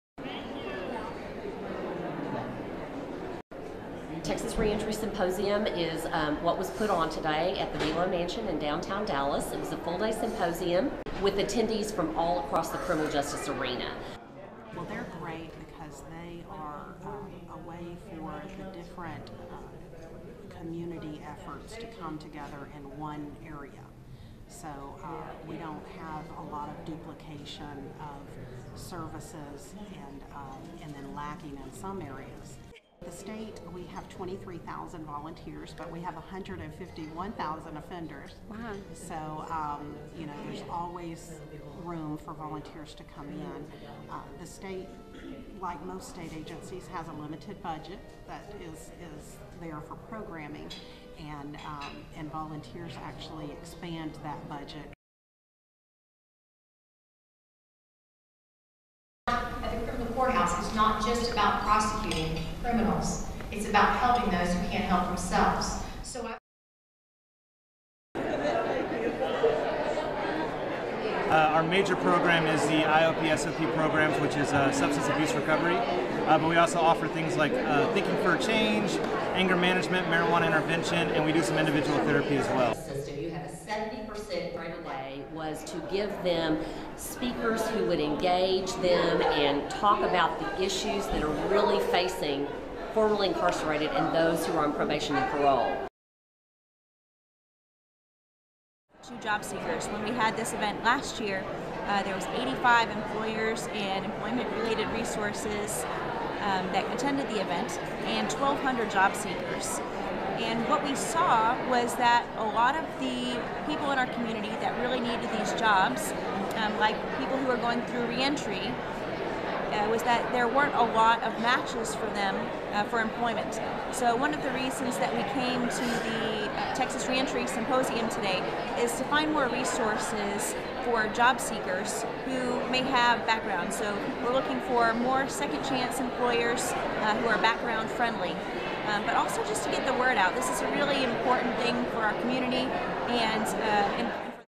Texas 2015 Reentry Symposium
Crime reduction has been a focus for the city of Dallas. As a result of efforts by the city and the Dallas Police Department in 2014, Dallas had a 11% decrease of crime for the 11th consecutive year. But what happens when ex-offenders reenter society. How do you ensure success of reentry and reduce recidivism?